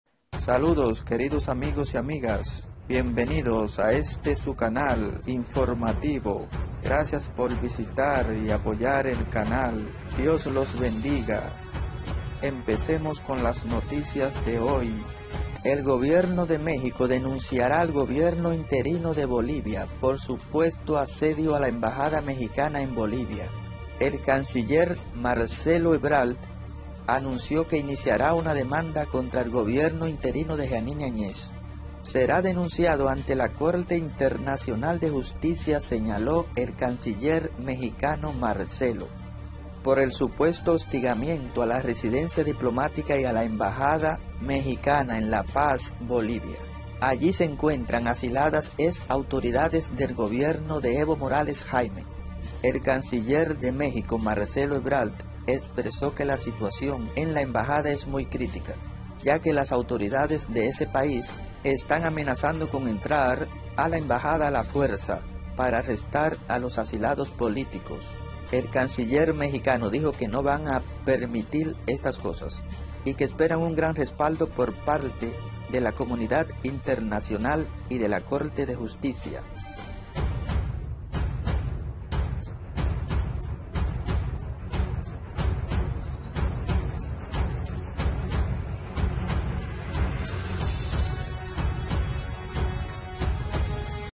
MEXICO Noticias hoy VIERNES 27 DICIEMBRE' Denunciaremos al GOBIERNO de BOLIVIA!
MEXICO DENUNCIARA AL GOBIERNO INTERINO DE BOLIVIA, POR SUPUESTAMENTE (ASEDIO) EN LA EMBAJADA MEXICADA EN BOLIVIA.

BOLIVIA NOTICIAS

MEXICO NOTICIAS HOY

ULTIMAS NOTICIAS DE MEXICO

ULTIMAS NOTICIAS DE BOLIVIA